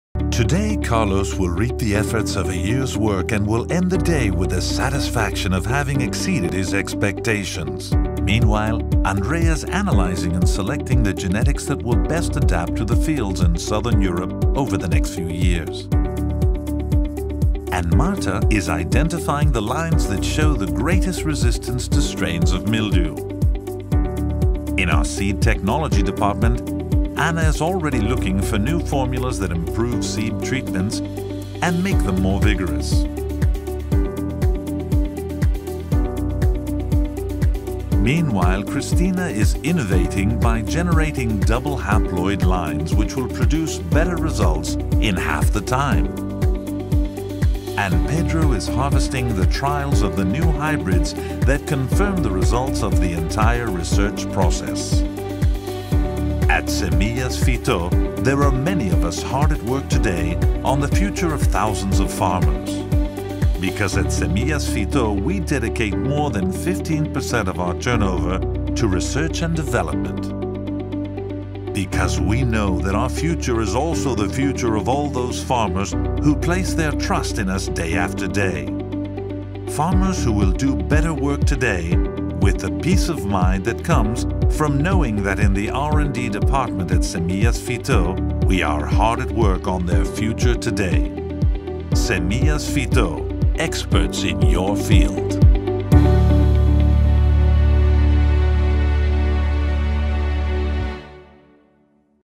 Semillas Fitóда илмий-тадқиқот ишлари компания ривожининг асосидир
Биз бюджетимизнинг 15% дан ортиғини ва ишчи кучимизнинг 39% ни глобал изланишлар ва деҳқончиликни ривожлантиришга сарфлаймиз.📈

Видео манзур келса агар қолганларга ҳам улашинг😊
Если вам понравилось видео, поделитесь им с другими😊